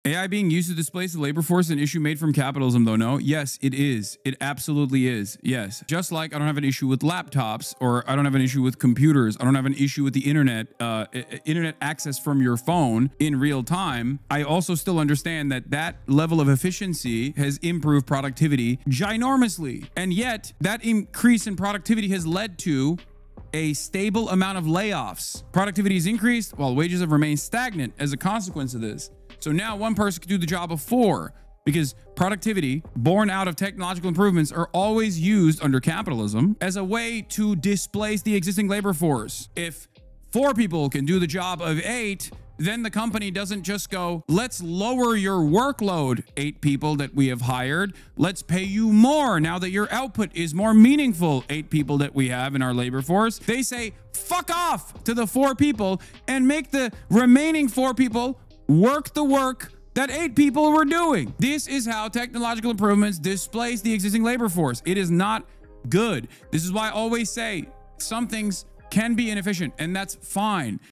HOW TECHNOLOGY DISPLACE EXISTING LABOR FORCE!
Hasan talks about AI and how technology affects the existing labor force